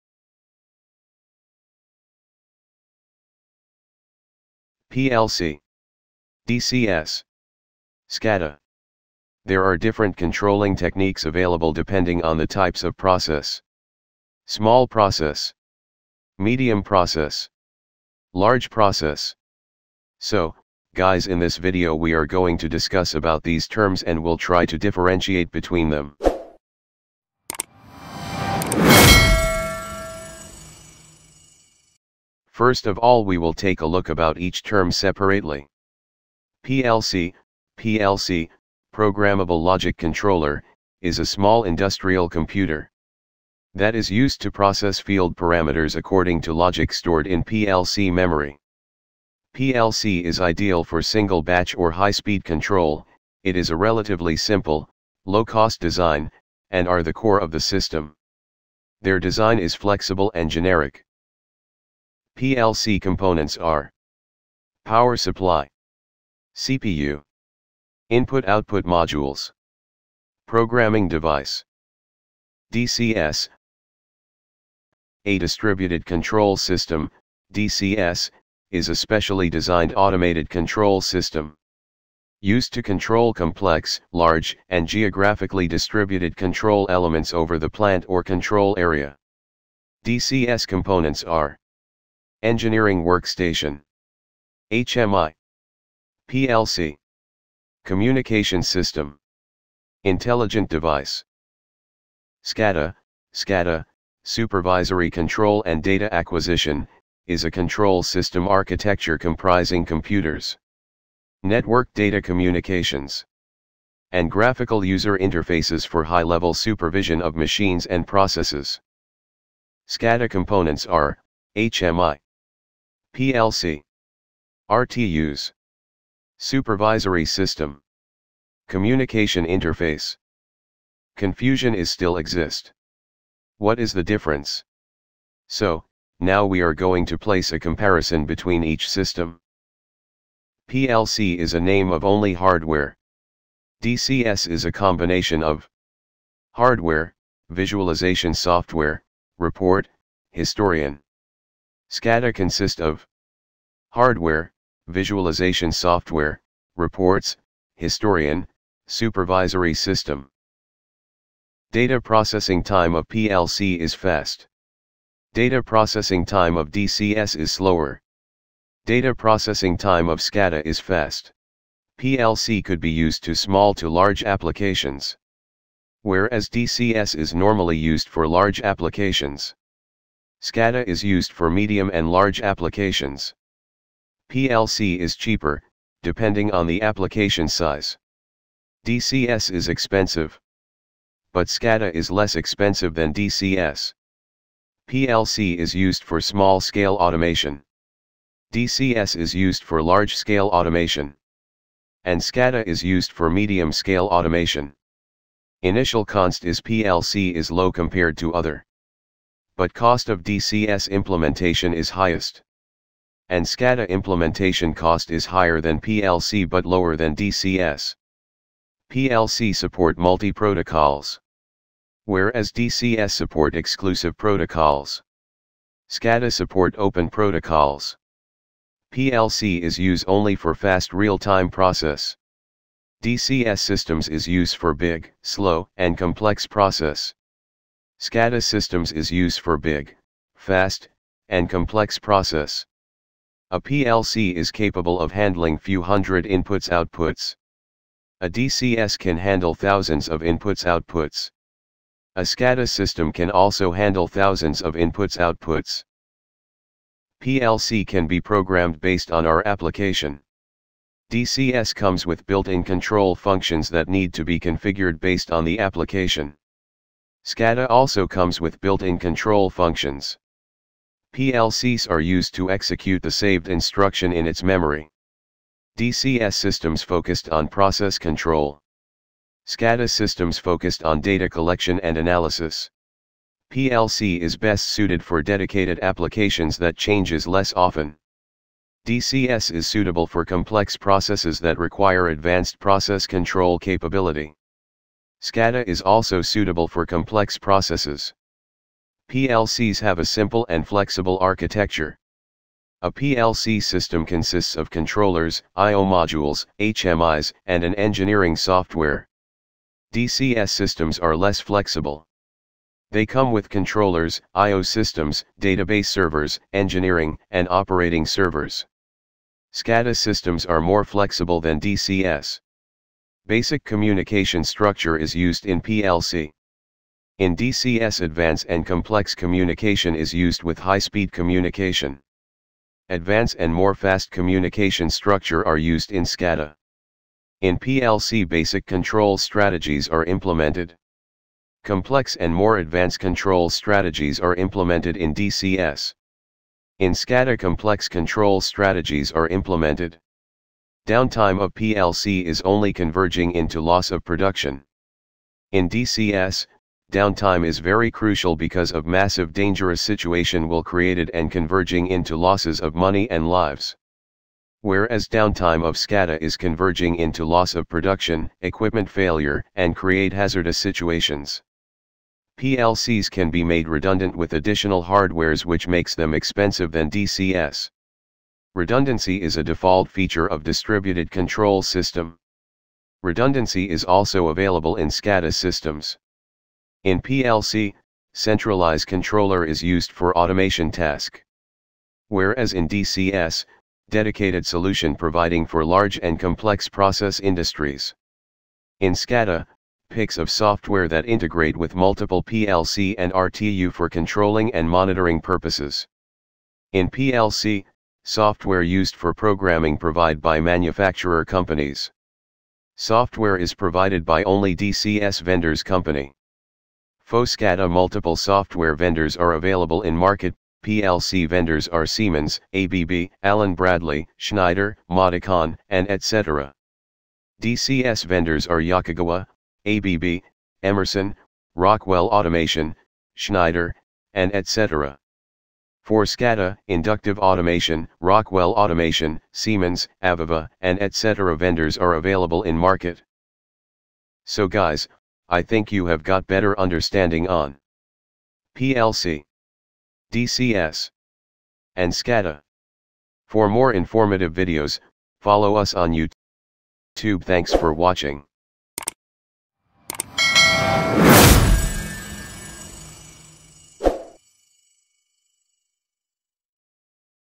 what is the difference between PLC, DCS and SCADA
Guys in this video we are going to discuss about these terms and will try to differentiate between them